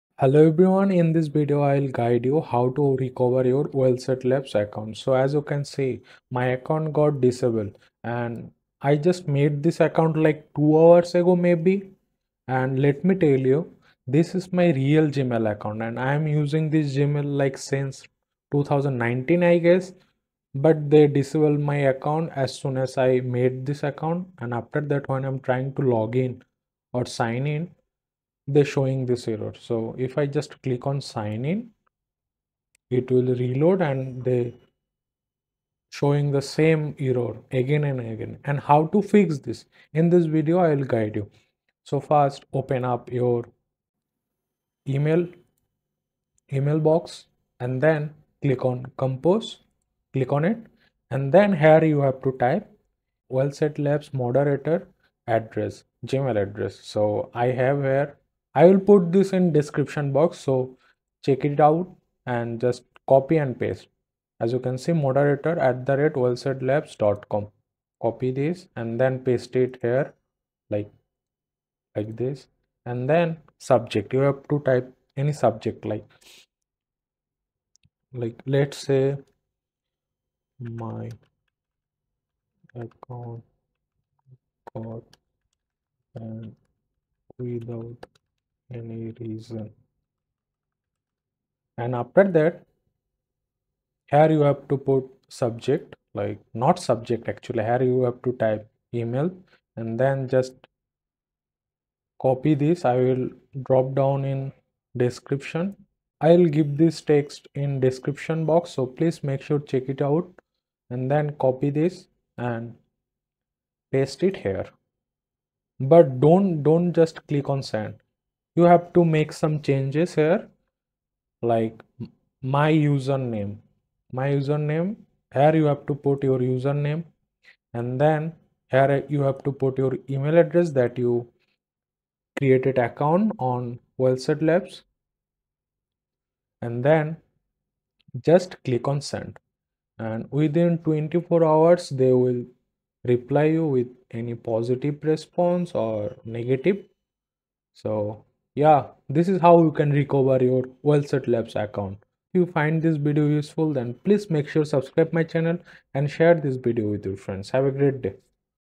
WellSaid Labs Account disabled or temporarily blocked | How to Recover WELLSAIDLABS Account
In this video I Will show you How to Recover Your Wellsaid Labs Account if your account Got disabled or Temporarily Blocked

Dear Wellsaid Labs Support Team,

I hope this email finds you well. I am writing to you with regards to my Wellsaid Labs account, which has been temporarily disabled and blocked. I am reaching out to request your assistance in recovering access to my account.

Unfortunately, I am currently unable to access my account due to the temporary block, and I am unsure of the reason behind this action. I rely on my Wellsaid Labs account for [mention the purposes, such as accessing important documents, communication, or utilizing specific features], and its unavailability has caused me significant inconvenience.

I kindly request that you review the status of my account and provide me with any necessary steps to regain access. If there are any issues or violations associated with my account, I am more than willing to cooperate and address them promptly to ensure compliance with Wellsaid Labs' policies and terms of service.

I understand the importance of maintaining the security and integrity of your platform, and I assure you that I am committed to adhering to all guidelines and regulations. I sincerely apologize for any inconvenience or misunderstanding that may have led to the temporary block, and I am eager to resolve this matter as soon as possible.

Please let me know if there are any additional details or information required from my end to facilitate the account recovery process. Your prompt attention to this matter would be greatly appreciated.

Thank you for your understanding and assistance. I look forward to hearing from you soon.

Warm regards,

We help make your life easier online!
You can expect great & informative tutorials, tips, guide & more from How To Explain Quick.